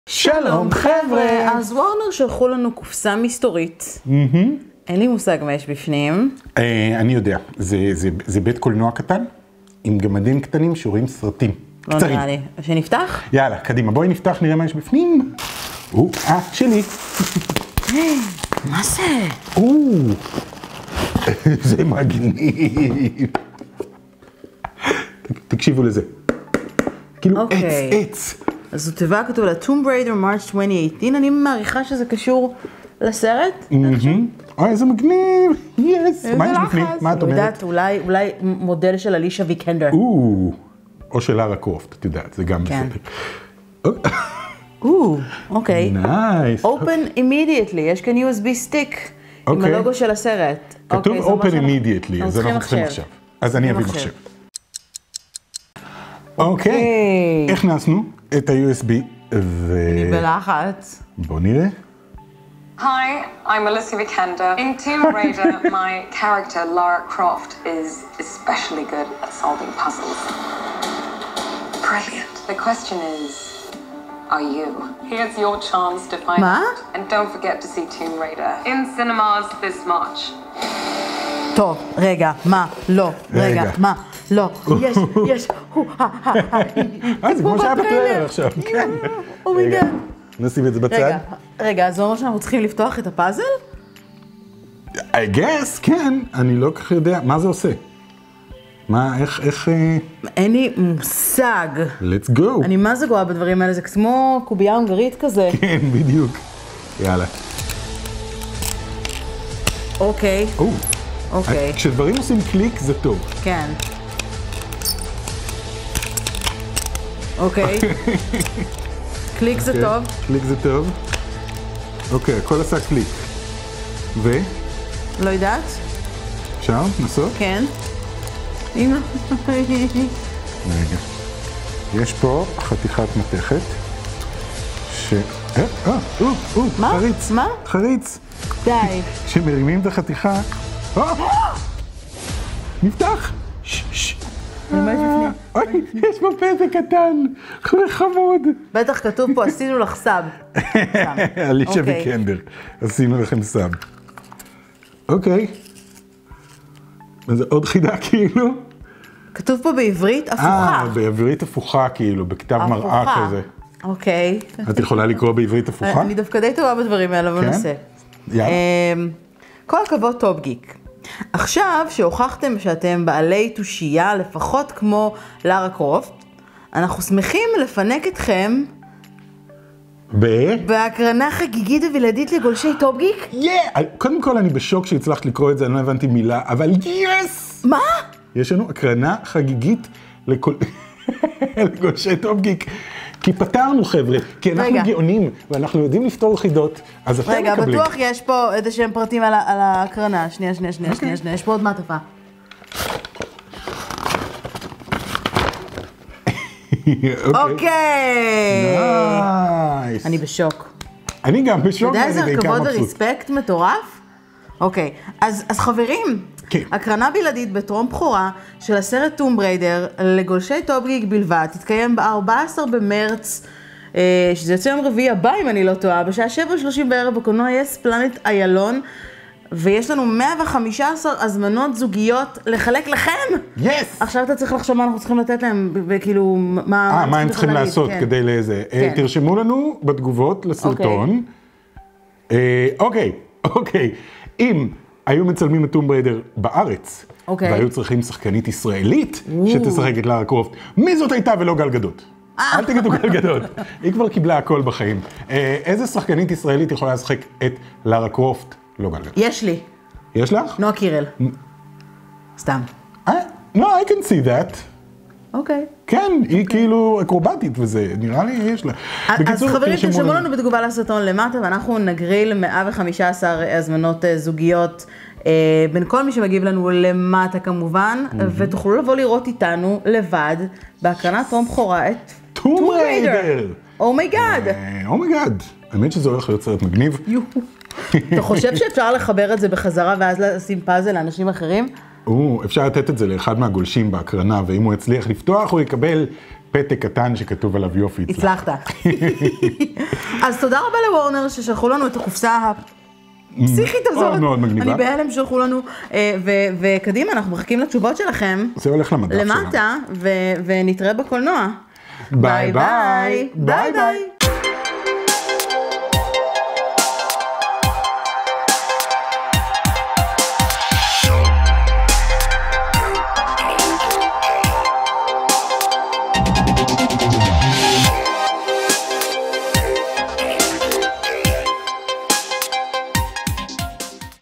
לארה קרופט שלחה לנו פאזל!!!
וורנר שלחו לנו קופסא מסתורית והיינו בשוק לגלות מה יש בתוכה.
צפו בסרטון המלא ותגלו כיצד תוכלו לקבל כרטיס זוגי להקרנת טרום בכורה פרטית של "טומב ריידר"...

Tomb Raider, starring Alicia Vikander, hits theaters March 16, 2018!

TOMB RAIDER and LARA CROFT are registered trademarks or trademarks of Square Enix Ltd.